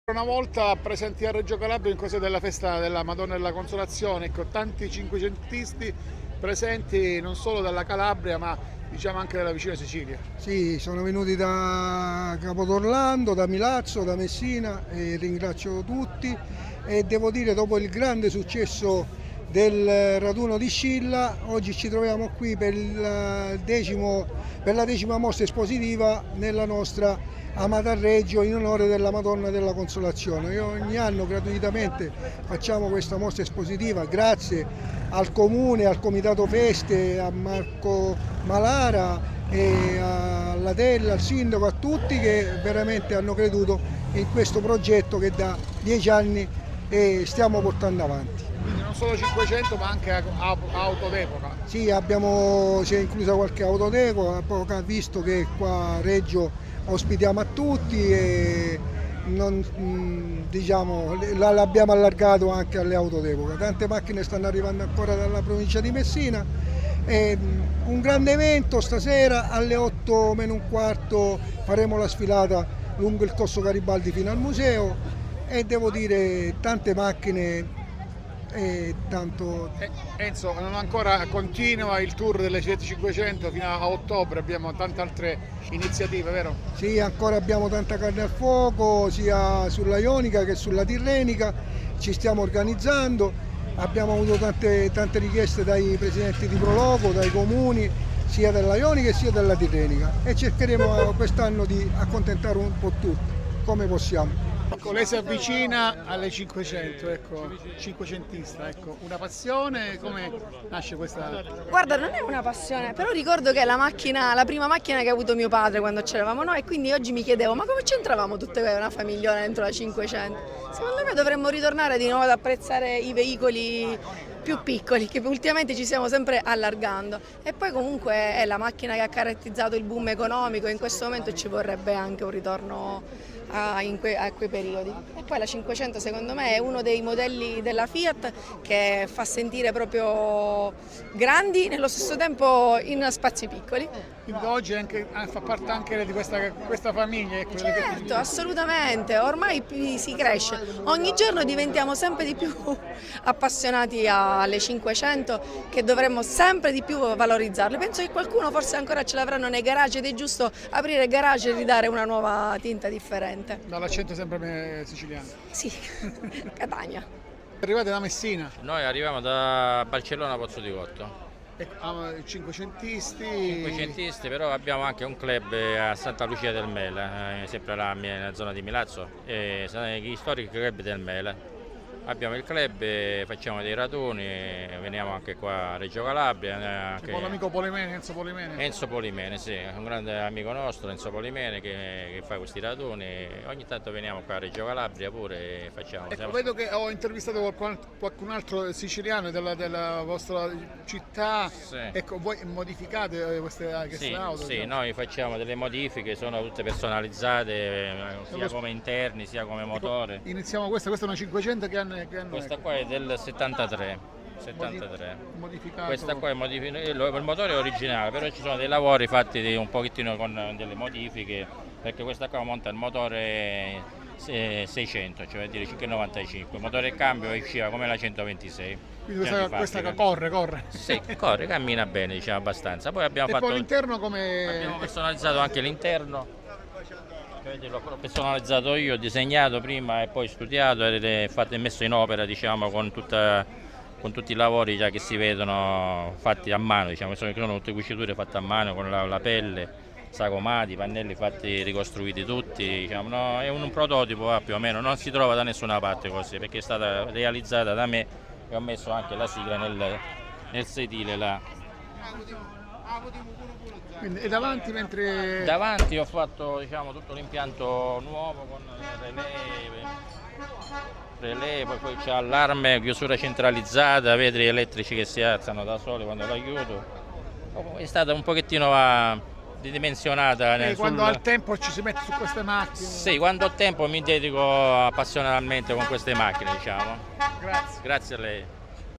Raduno 500 alla festa della madonna della consolazione
Reggio Calabria | tante 500 d' epoca provenienti dalla Calabria e non solo, per il raduno tenuto a Reggio in onore della festa della madonna della consolazione